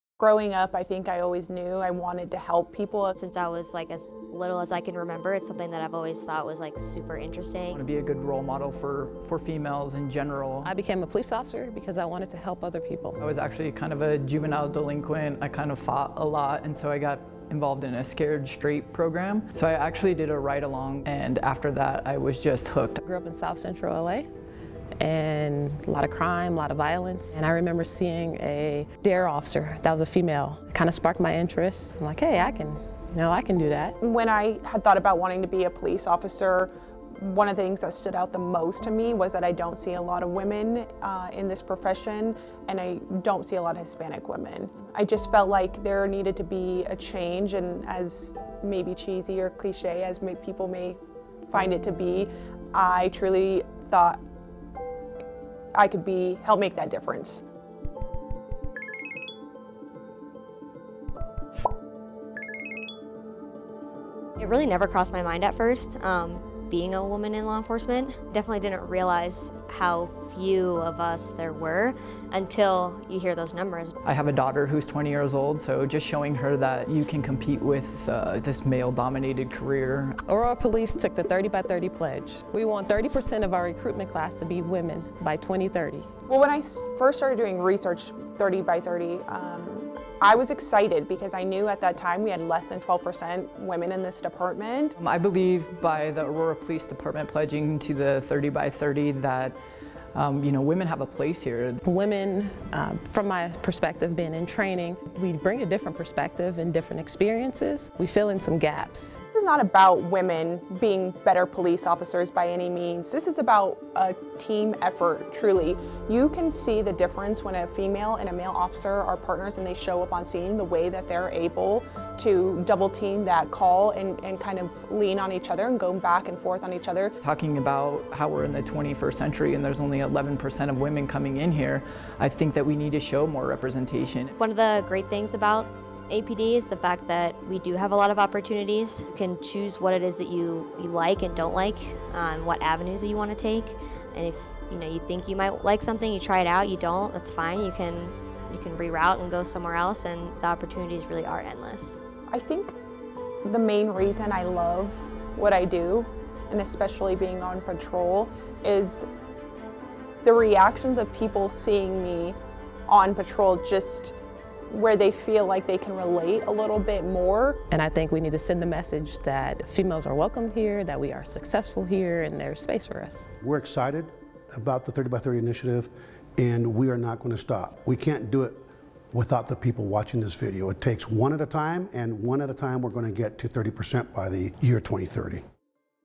APD Joins the 30 by 30 Initiative
By the year 2030, APD pledges to have a staff made up of 30% women.